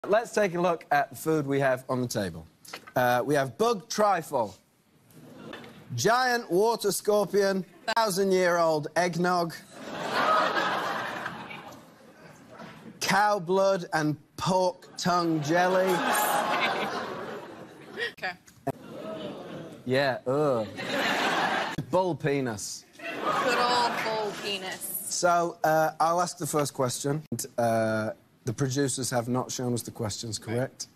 Uncovering MindBlowing Delicacies Discover the Shocking Secrets of Exotic Foods
The video grabs attention with an intriguing hook and showcases unusual foods, aligning with the trend of exploring exotic cuisine. However, it lacks a clear transition and conclusion, and could benefit from a more engaging delivery and personal connection to captivate viewers further.

Let's take a look at food we have on the table We have bug trifle Giant water scorpion Thousand year old eggnog Cow blood and pork tongue jelly Okay Yeah uh And bull penis Good old bull penis So I'll ask the first question And the producers have not shown us the questions correct